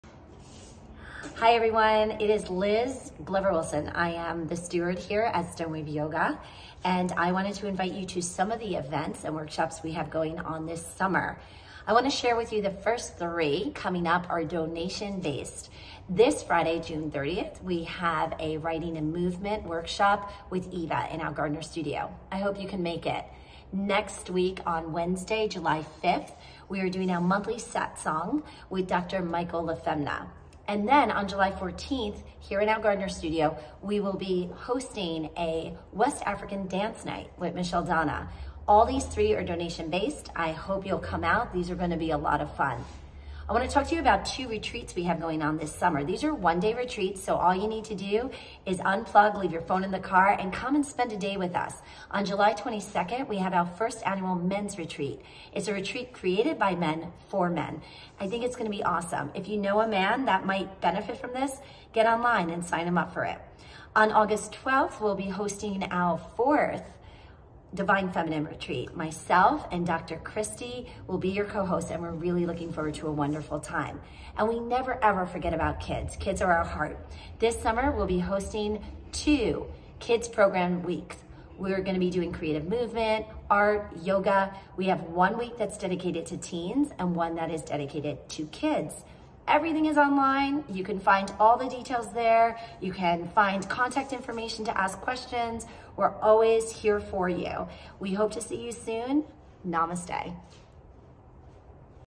Harness Creativity
Stone Wave Yoga, Founder and Steward, Liz Glover Wilson shares upcoming events happening at Stone Wave Yoga in Gardiner and Poughkeepsie, NY.